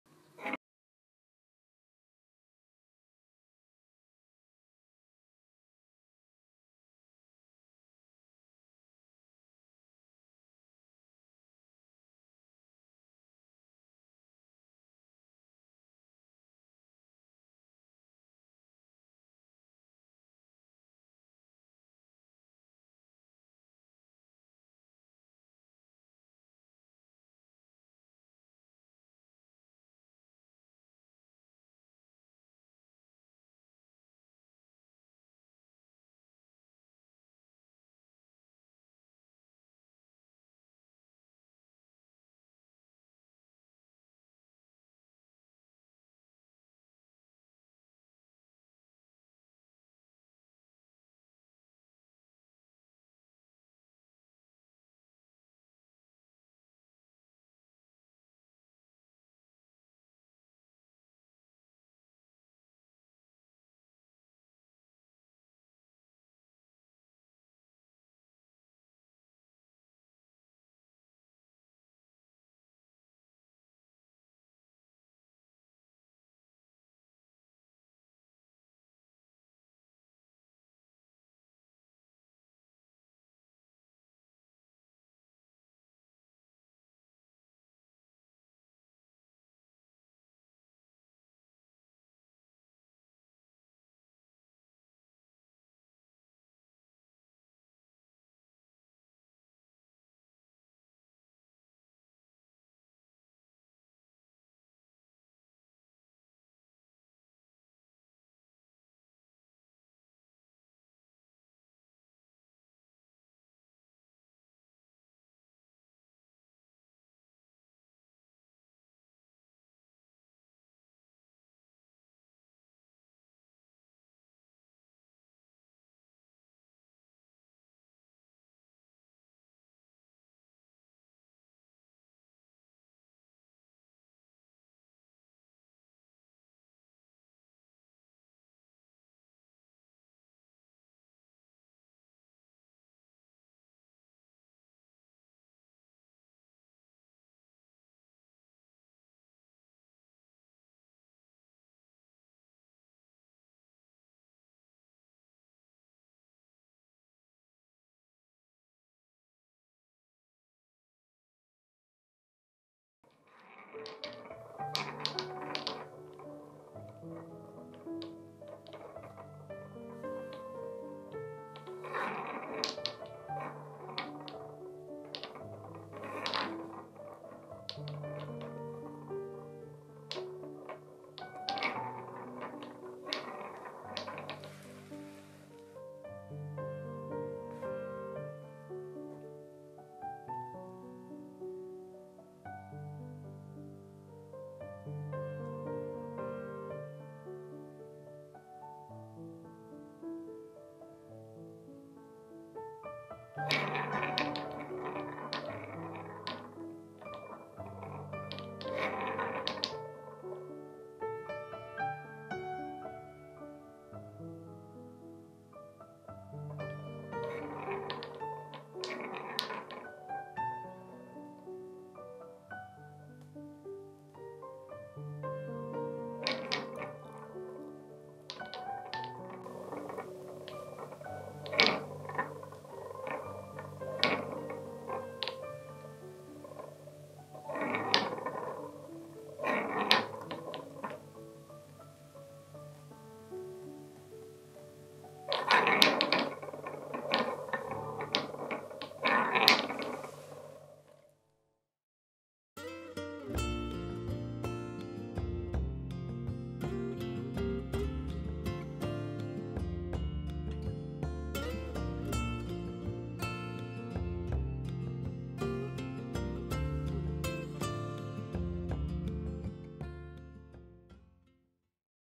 This toy has the magical power to put a kitten to sleep
・Name        Coco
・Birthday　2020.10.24
・Cat breed  Munchkin (Short legged)
・Sex             Male

・Name　　 Mimi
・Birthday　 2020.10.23
・Cat breed  British Shorthair
・Sex　　　 Female